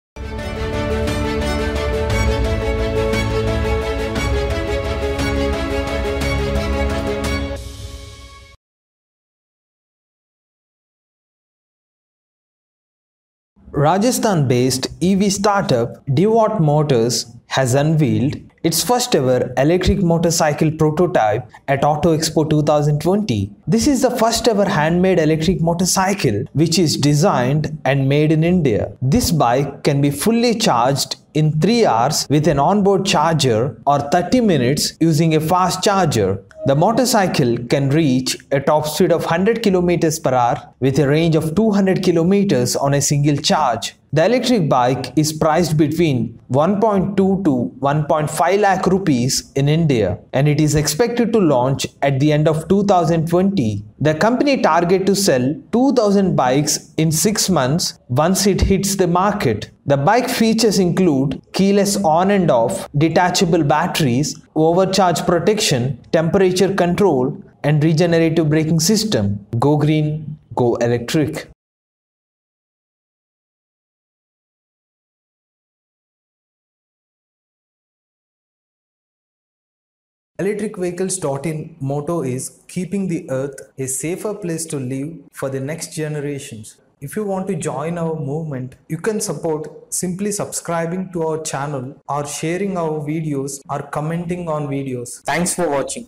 India's First Hand Made Electric Motorcycle | Devot Motors
Devot Motors has unveiled India's First Hand Made Electric Motorcycle at Auto Expo 2020. It is expected to price between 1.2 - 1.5 lakhs ready for launch at the end of 2020.